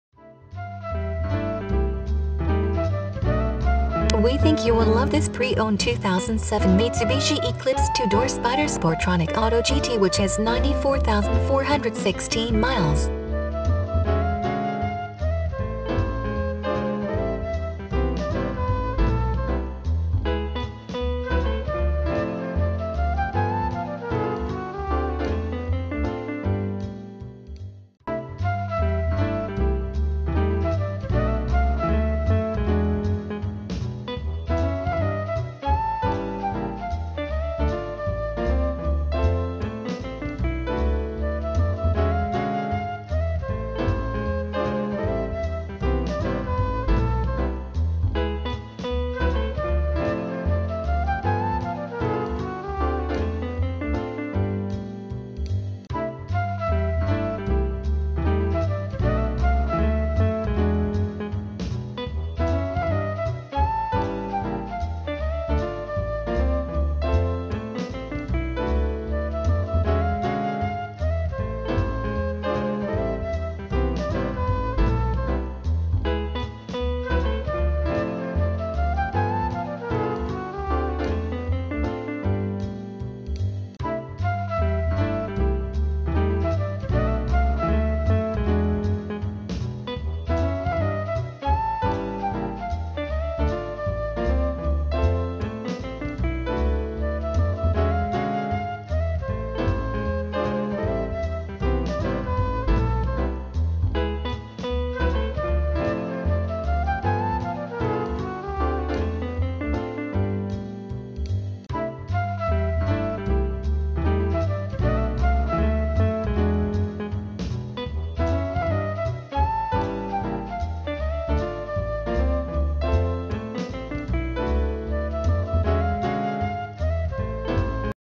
2007 MITSUBISHI ECLIPSE SPYDER SPORTRONIC AUTO GT CONVERTIBLE
2007 MITSUBISHI Eclipse 2 Door Spyder Sportronic Auto GT
Stock Number: 13801E
VIN: 4A3AL35T27E030367

We think you will love this PRE-OWNED 2007 MITSUBISHI Eclipse 2 Door Spyder Sportronic Auto GT which has 94416 miles. It comes fully equipped with Abs (4-Wheel), Air Conditioning, Alloy Wheels, and many other features.Please call us for a walk around description of the vehicle or to schedule a test drive.